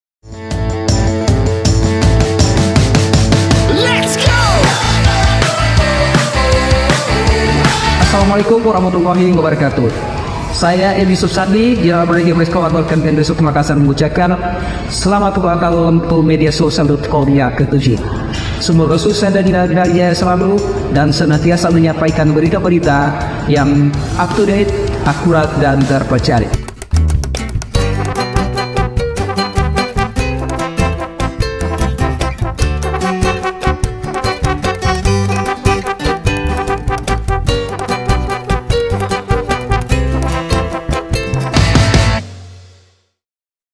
Yusuf Sandy - General Manager Maxone Hotel & Resort Makassar | Anniversary 7th Mediasulsel.com
Yusuf Sandy, General Manager Maxone Hotel & Resort Makassar beri ucapan Selamat Ulang Tahun ke-7 untuk Mediasulsel.com | Media Era Digital